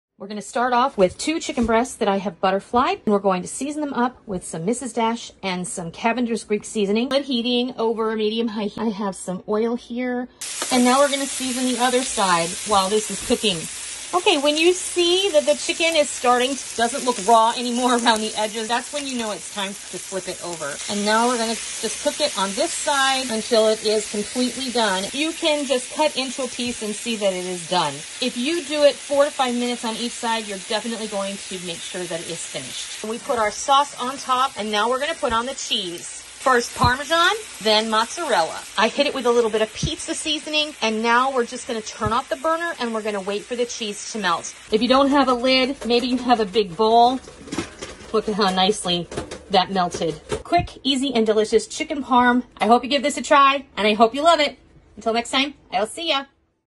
SKILLET CHICKEN PARM!
Today I am starting a new series where I will be sharing quick and easy recipes for new cooks!  This is brought on by the fact that both my daughters are starting their life journey with new jobs and serious beaus and their future is very bright!  My oldest has great ability in the kitchen, my youngest, not so much.  She is now figuring out that if she wants to eat she needs to know how to cook some simple but delicious things for herself.  With that idea in mind, I am going to add some of those to the catalog.  I am starting with this delicious, skillet chicken parmesan.  A few ingredients, from the fridge and pantry can make this delicious meal in under a half an hour and it is so satisfying and comforting!